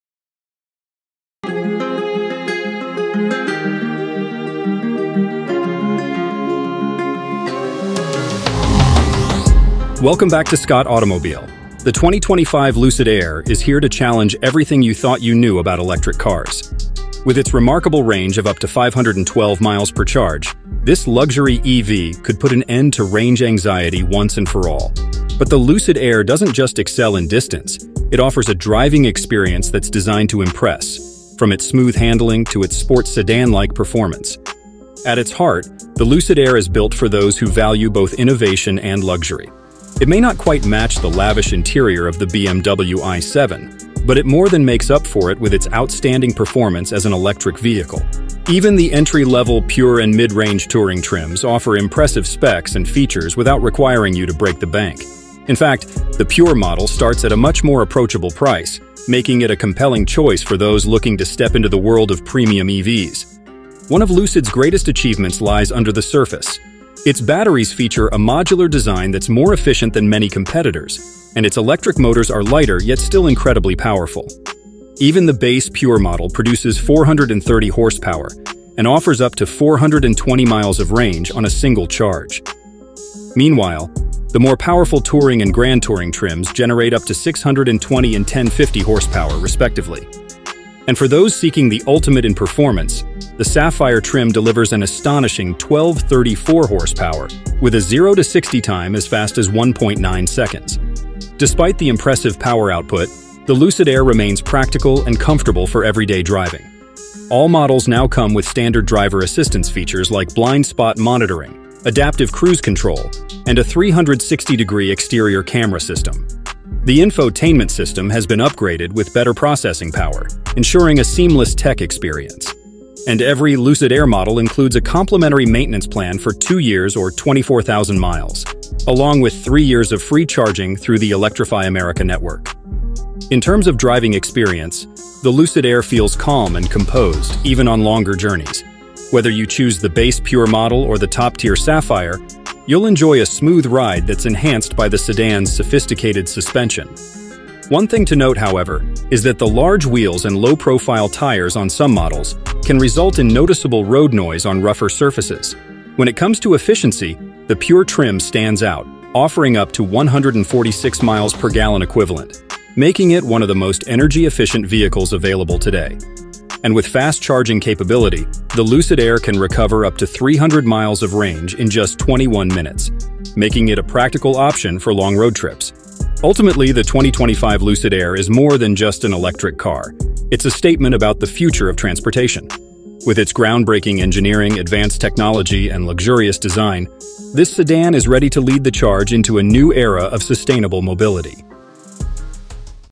2025 Lucid Air - Breaking the 500-Mile EV Range Barrier!
2025 Lucid Air - Breaking the 500-Mile EV Range Barrier!

The 2025 Lucid Air is here to challenge everything you thought you knew about electric cars. With its remarkable range of up to 512 miles per charge, this luxury EV could put an end to range anxiety once and for all. But the Lucid Air doesn’t just excel in distance; it offers a driving experience that’s designed to impress, from its smooth handling to its sports sedan-like performance.

At its heart, the Lucid Air is built for those who value both innovation and luxury. It may not quite match the lavish interior of the BMW i7, but it more than makes up for it with its outstanding performance as an electric vehicle. Even the entry-level Pure and mid-range Touring trims offer impressive specs and features without requiring you to break the bank. In fact, the Pure model starts at a much more approachable price, making it a compelling choice for those looking to step into the world of premium EVs.

One of Lucid’s greatest achievements lies under the surface. Its batteries feature a modular design that’s more efficient than many competitors, and its electric motors are lighter yet still incredibly powerful. Even the base Pure model produces 430 horsepower and offers up to 420 miles of range on a single charge. Meanwhile, the more powerful Touring and Grand Touring trims generate up to 620 and 1050 horsepower, respectively. And for those seeking the ultimate in performance, the Sapphire trim delivers an astonishing 1234 horsepower with a 0 to 60 time as fast as 1.9 seconds.

Despite the impressive power output, the Lucid Air remains practical and comfortable for everyday driving. All models now come with standard driver assistance features like blind-spot monitoring, adaptive cruise control, and a 360-degree exterior camera system. The infotainment system has been upgraded with better processing power, ensuring a seamless tech experience. And every Lucid Air model includes a complimentary maintenance plan for two years or 24,000 miles, along with three years of free charging through the Electrify America network.

In terms of driving experience, the Lucid Air feels calm and composed, even on longer journeys. Whether you choose the base Pure model or the top-tier Sapphire, you’ll enjoy a smooth ride that’s enhanced by the sedan’s sophisticated suspension. One thing to note, however, is that the large wheels and low-profile tires on some models can result in noticeable road noise on rougher surfaces.

When it comes to efficiency, the Pure trim stands out, offering up to 146 miles per gallon equivalent, making it one of the most energy-efficient vehicles available today. And with fast-charging capability, the Lucid Air can recover up to 300 miles of range in just 21 minutes, making it a practical option for long road trips.

Ultimately, the 2025 Lucid Air is more than just an electric car—it’s a statement about the future of transportation. With its groundbreaking engineering, advanced technology, and luxurious design, this sedan is ready to lead the charge into a new era of sustainable mobility.